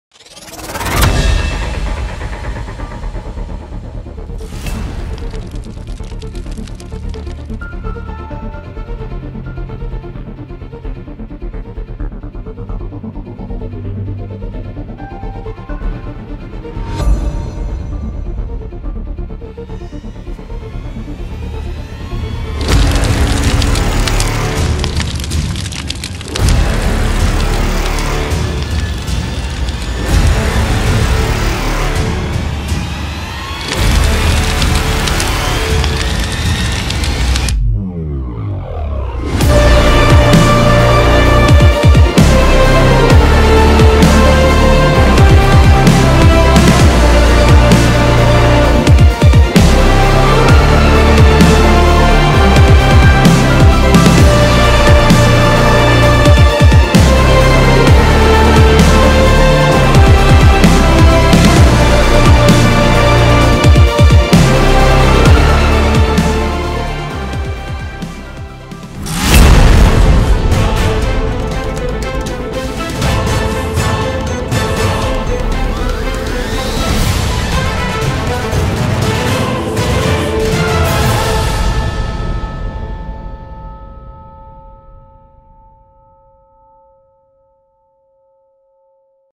SecondLife Network 2.0 - Subota u 20:00h
Posetite  nas u Subotu u 20:00h na otvaranju SecondLife Network Servera :)
IGN : VanjaTehG0D
Faction : Aloha/Ruluru (Spojeni Fac)
Poz ljudi dodjite svi na open second life-a!
RECRUITAMO PLAYERE ZA FACTION JAVITE SE U COMM!
Pozdrav!